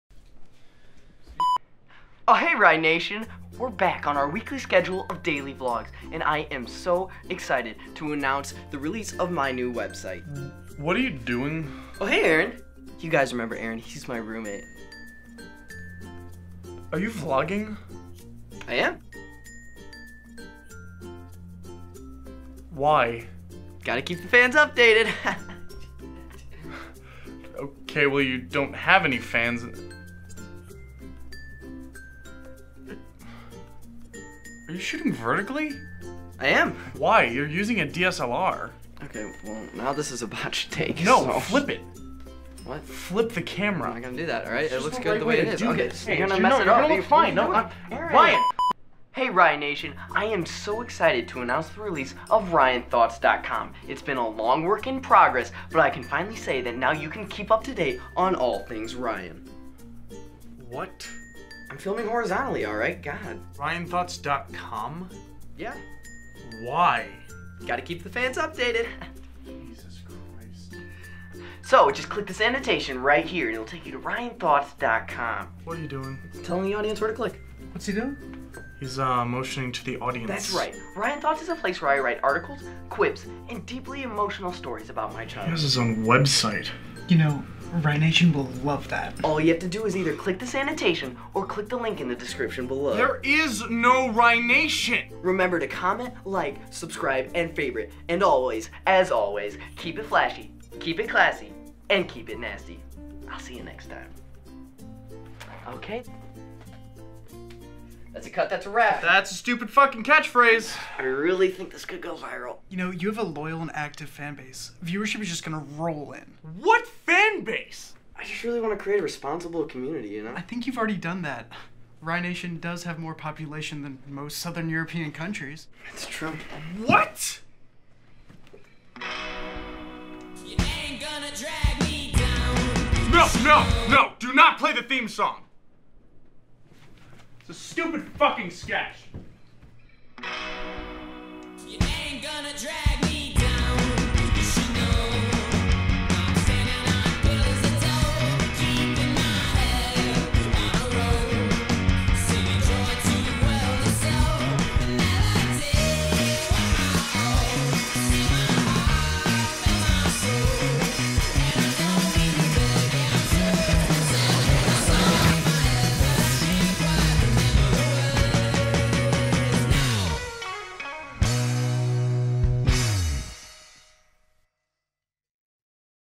Ryan Thoughts Dot Com
Ryan urges the audience to check out his new website. Aaron's not having it.

Guest Starring: Austin Merrill
Shot By: Alan Dembek
Sound By: Adam Carl